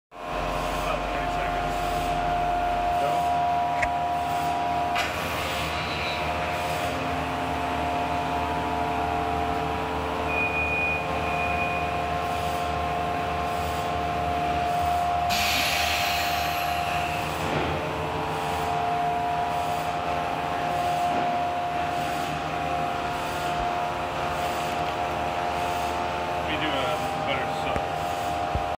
Promarks DC 800 Double Chamber Vacuum Sealer #5870
for more information about vacuum sealer machines. Promarks DC-800 Double Chamber, 38.5" X 31" X 8" chambers, Built in Busch 100 CFM, 3 HP vacuum pump, 220V/3PH/60HZ, (4) 31.5" seal bars, 24" between seal bars, Stainless steel construction, Microprocessor controls.